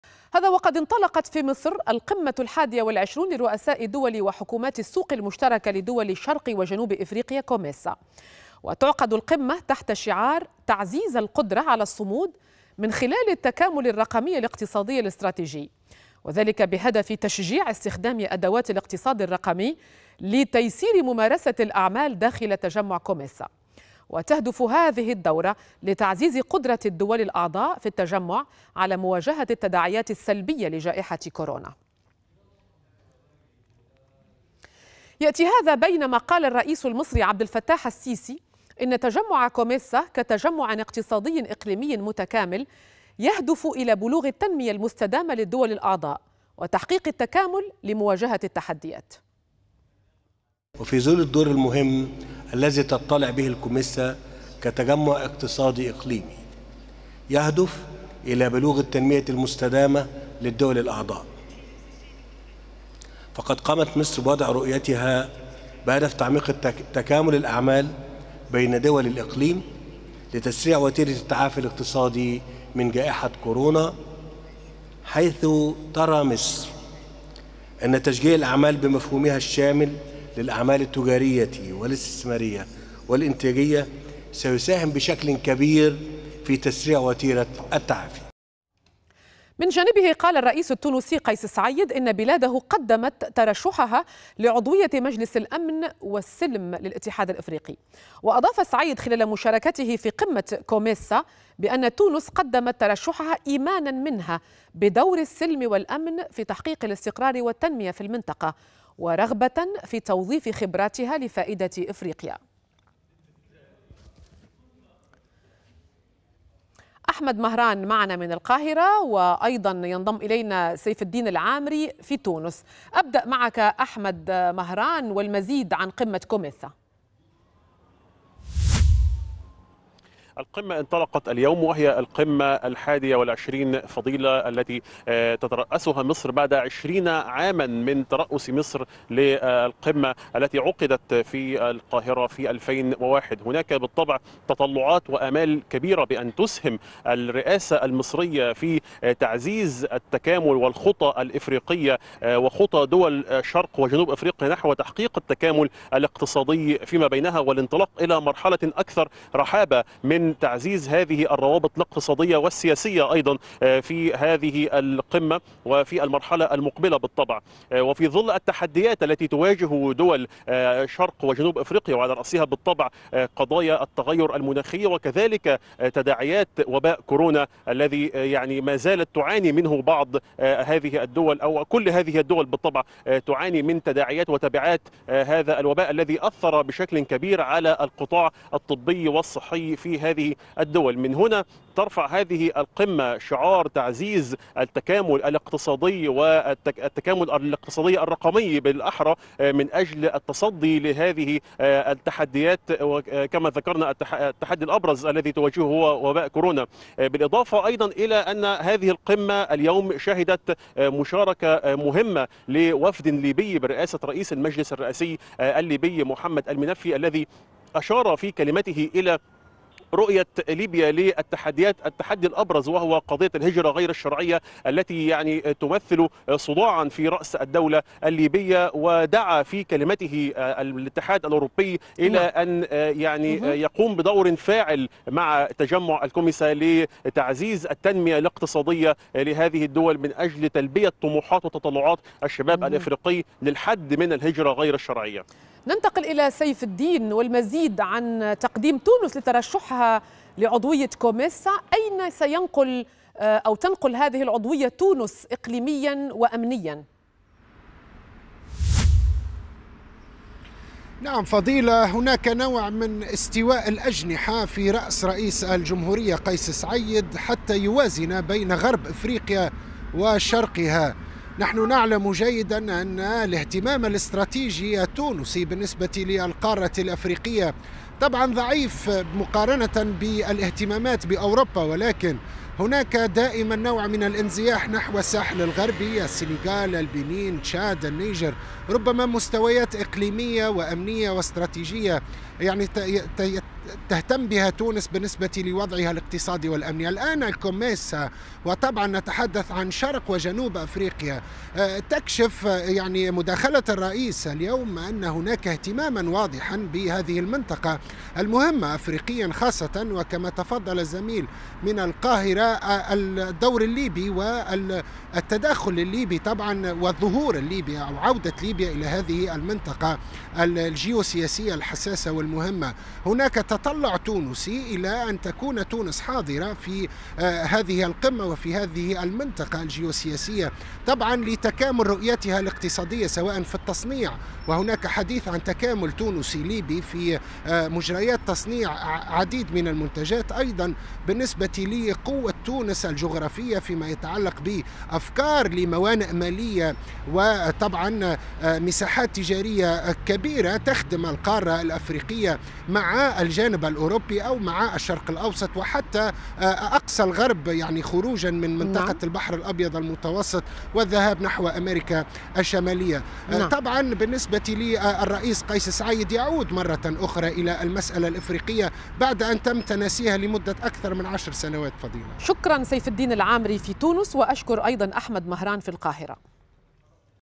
انطلاق القمة الـ21 لدول شرق وجنوب إفريقيا" الكوميسا"
انطلقت في مصر، القمةُ الحادية والعشرون لرؤساء دول وحكومات السوق المشتركة لدول شرق وجنوب إفريقيا "الكوميسا".وتُعقد القمةُ تحت شعار "تعزيز القدرة على الصمود من خلال التكامل الرقمي الاقتصادي الاستراتيجي"، وذلك بهدف تشجيع استخدام أدوات الاقتصاد الرقمي لتيسير ممارسة الأعمال داخل تجمع الكوميسا.
وتهدف هذه الدورة لتعزيز قدرة الدول الأعضاء في التجمع، على مواجهة التداعيات السلبية لجائحة كورونا.من جانبه قال الرئيسُ التونسي قيس سعيد إن بلاده قدّمت ترشّحها لعضوية  مجلس السلم والأمن للاتحاد الإفريقي. وأضاف سعيد خلال مشاركته في قمة الكوميسا أن تونس قدمت ترشحها إيمانا منها بدور السّلم والأمن في تحقيق الاستقرار والتنمية في المنطقة ورغبةً في توظيف خبراتها لفائدة إفريقيا.
السيسي#
مصر#
عبدالفتاح_السيسي#
الكوميسا#